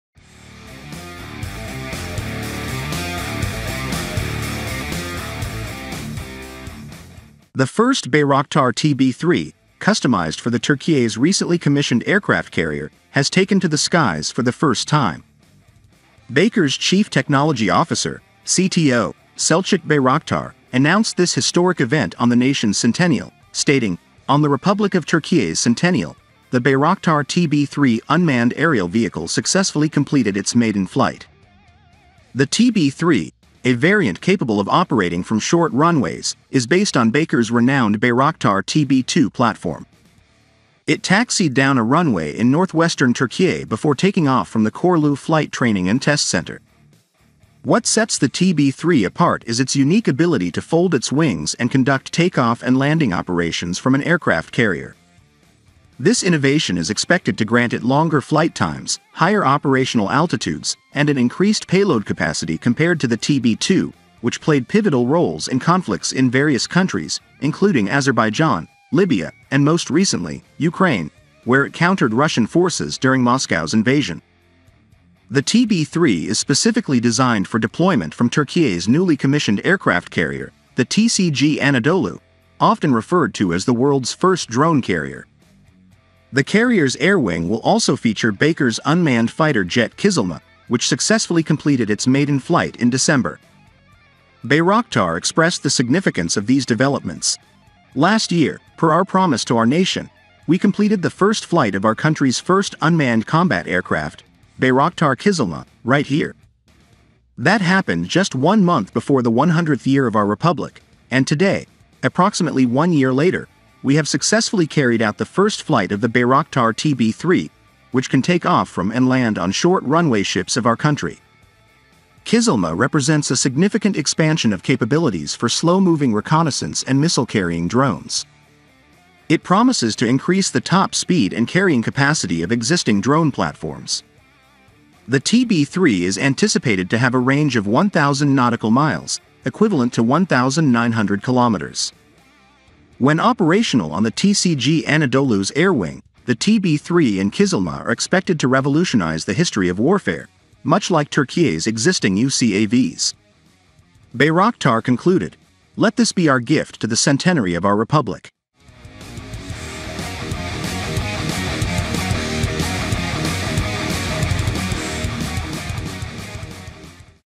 Türkiye's new Bayraktar TB3 combat drone has flown
The first Bayraktar TB3, customized for the Türkiye’s recently commissioned aircraft carrier, has taken to the skies for the first time.

Baykar’s Chief Technology Officer (CTO), Selçuk Bayraktar, announced this historic event on the nation’s centennial, stating, “On the Republic of Türkiye’s centennial, the Bayraktar TB3 unmanned aerial vehicle successfully completed its maiden flight.”

The TB3, a variant capable of operating from short runways, is based on Baykar’s renowned Bayraktar TB2 platform. It taxied down a runway in northwestern Türkiye before taking off from the Çorlu Flight Training and Test Center.

The TB3 is specifically designed for deployment from Türkiye’s newly commissioned aircraft carrier, the TCG Anadolu, often referred to as the world’s first drone carrier. The carrier’s air wing will also feature Baykar’s unmanned fighter jet Kızılelma, which successfully completed its maiden flight in December.

Bayraktar expressed the significance of these developments: “Last year, per our promise to our nation, we completed the first flight of our country’s first unmanned combat aircraft, Bayraktar Kızılelma, right here. That happened just one month before the 100th year of our Republic, and today, approximately one year later, we have successfully carried out the first flight of the Bayraktar TB3, which can take off from and land on short-runway ships of our country.”

Kızılelma represents a significant expansion of capabilities for slow-moving reconnaissance and missile-carrying drones. It promises to increase the top speed and carrying capacity of existing drone platforms.

The TB3 is anticipated to have a range of 1,000 nautical miles, equivalent to 1,900 kilometers. When operational on the TCG Anadolu’s air wing, the TB3 and Kızılelma are expected to revolutionize the history of warfare, much like Türkiye’s existing UCAVs.